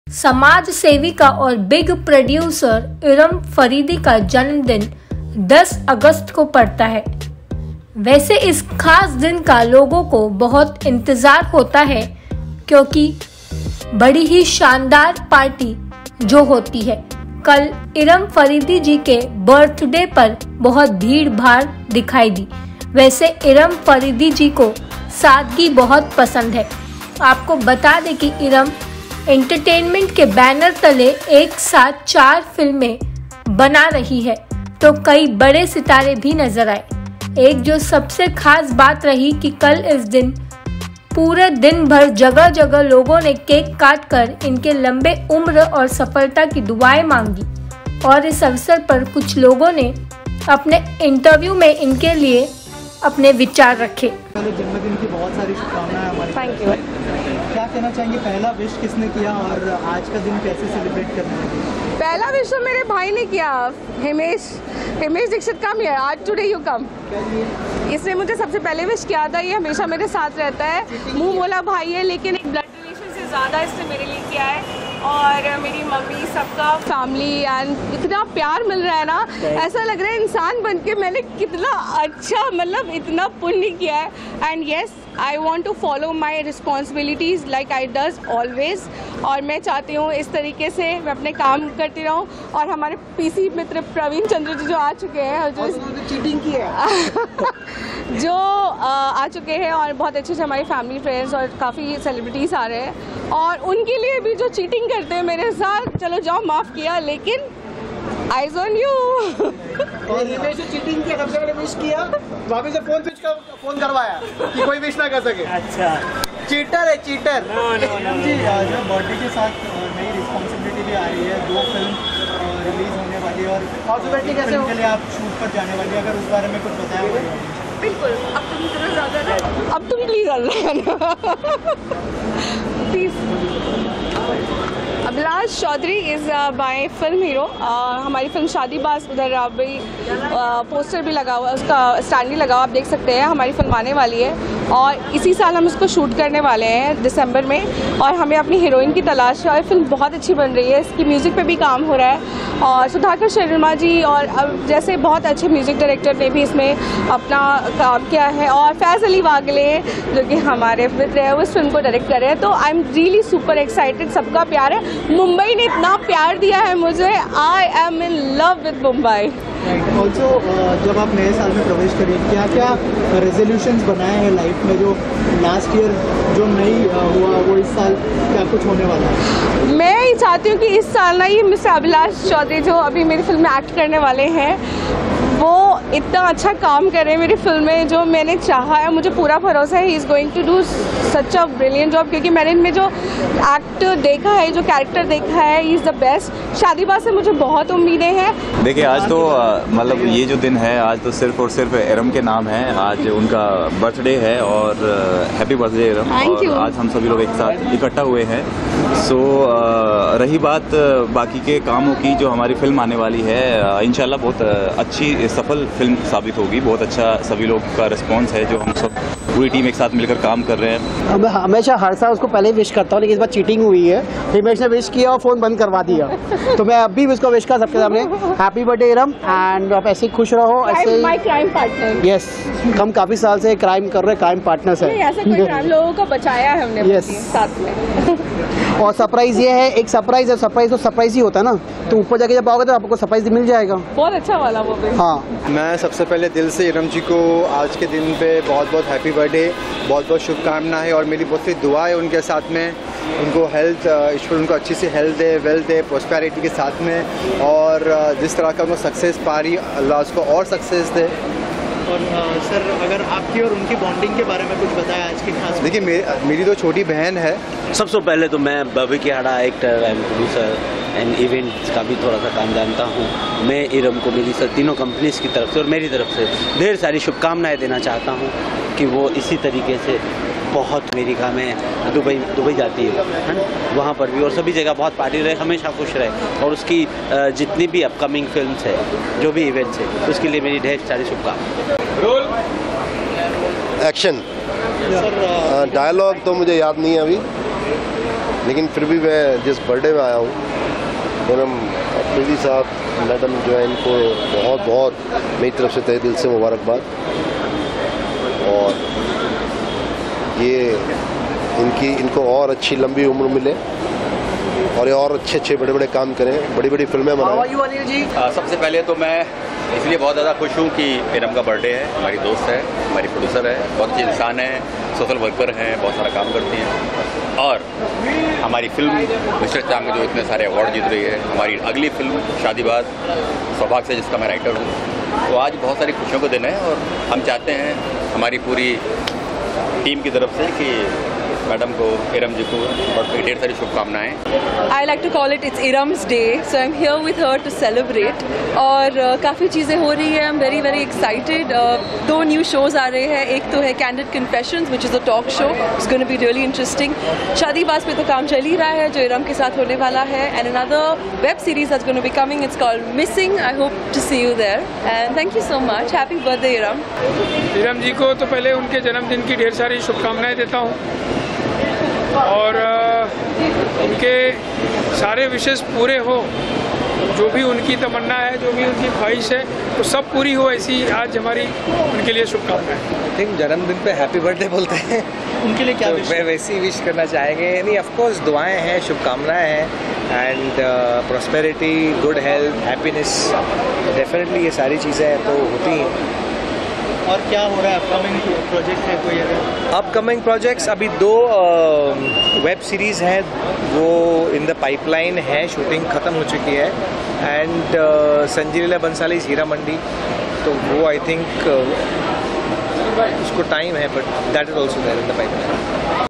Film producer Eram Faridi's Grand birthday celebrated in Mumbai, many celebrities attended
समाज सेविका और बिग प्रोडूसर इरम फरीदी ka जन्मदिन 10अगस्त को पड़ता है वैसे इस खास दिन ka लोगो बहुत इंतजार होता है क्युकि बड़ी है शानदार पार्टी जो होती है कल इरम फरीद जी के बर्थड़े पर बहुत भीड़ भाड़ दिखाई दिया वैसे इरम फरीदी जी को सादगी बहुत पसंद है
आपको बतादे की इरम इंटरटेनमेंट के बैनर तले एक साथ चार फिल्मे बन रही है  तो कई बड़े सितारे भी नजर आये  एक जो सबसे खास बात रही की कल इस दिन पूरा दिन भर जगह जगह लोगो ने केक काटकर इनके लम्बे उम्र और सफलता की दुआएं मांगी इस अवसर पर कुछ लोगो ने आपने इंटरव्यू मे इनके लिए आपने विचाररखे